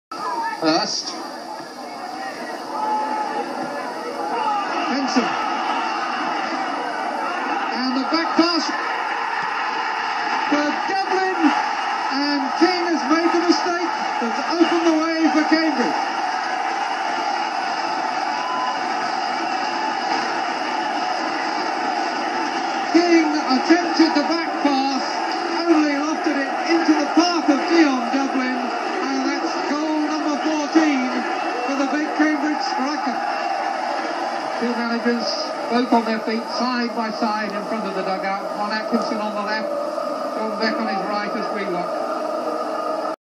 Dion Dublin 1st Goal. Cambridge United v Sheffield Wednesday. 16th Feb 1991
Dion Dublin Cambridge United. First goal Vs Sheffield Wednesday, 16th February 1991. Dion Dublin.